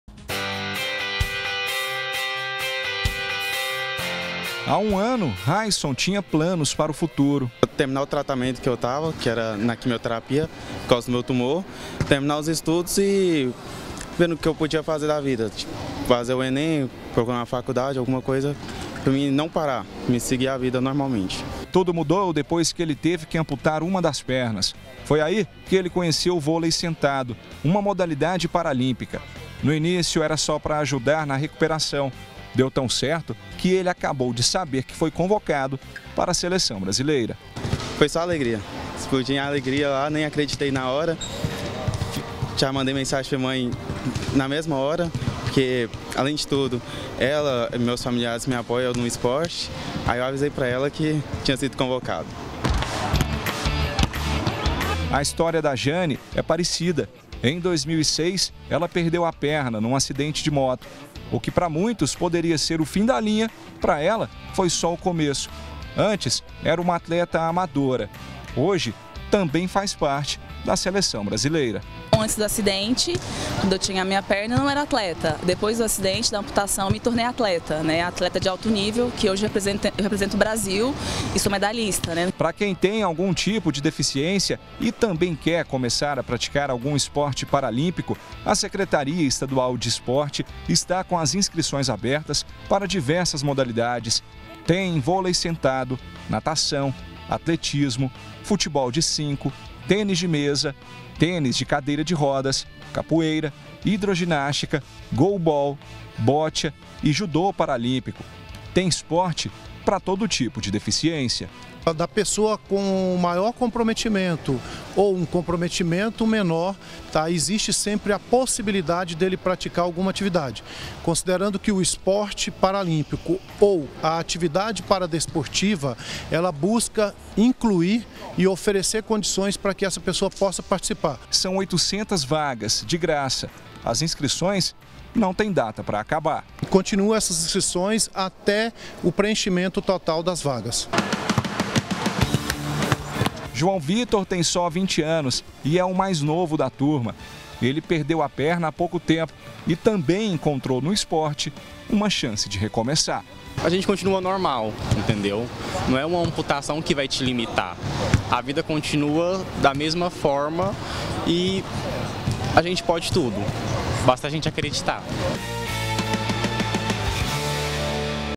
NOVAS TURMAS DE PARATLETAS EM GOIÂNIA
O sonho de ser atleta não depende apenas da condição física, mas sim de força de vontade, de garra, de determinação. Aqui em Goiânia, paratletas que treinam com apoio da Secretaria Estadual de Esporte são exemplos de superação. O projeto que está com inscrições abertas atende centenas de pessoas com deficiência.